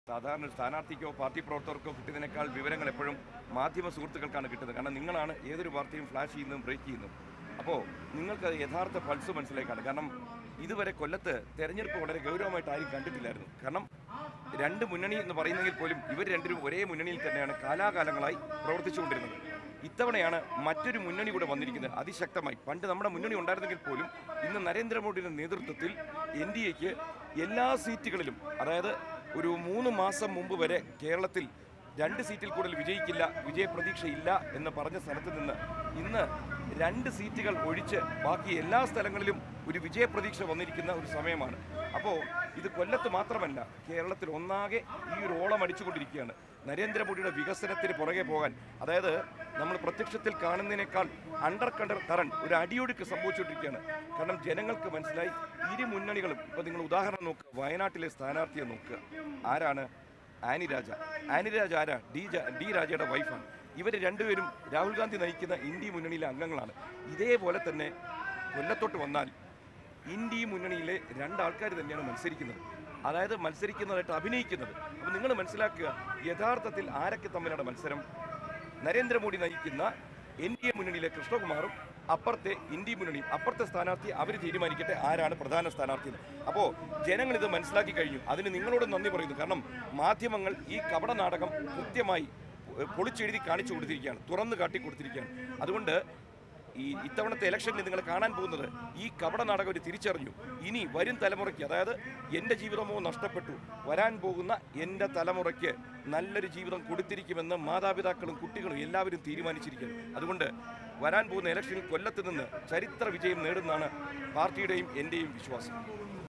''NDA മുന്നണിയും INDIA മുന്നണിയും തമ്മിലാണ് ഇവിടെ മത്സരം; ജനങ്ങൾ അത് മനസിലാക്കി കഴിഞ്ഞു''
NDA മുന്നണിയും INDIA മുന്നണിയും തമ്മിലാണ് ഇവിടെ മത്സരം; ജനങ്ങൾ അത് മനസിലാക്കി കഴിഞ്ഞു - കൃഷ്ണകുമാർ ജി

Happy viewing!